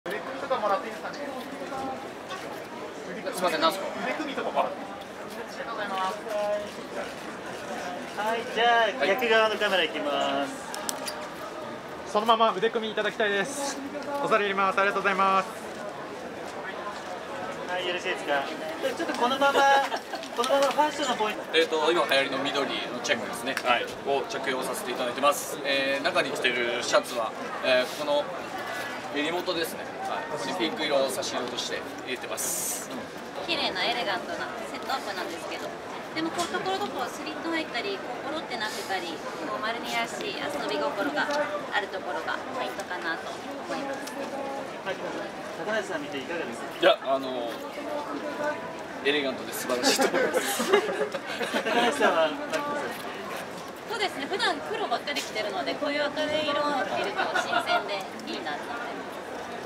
槙野智章&高梨臨 夫妻、突然の爆笑に仲良しすぎる雰囲気ダダ漏れ！
元サッカー日本代表の槙野智章と妻でモデルの高梨臨が1日、東京・国立代々木競技場 第二体育館で行われたファッションショー「MARNI FALL WINTER 2023 FASHION SHOW IN TOKYO」に登場した。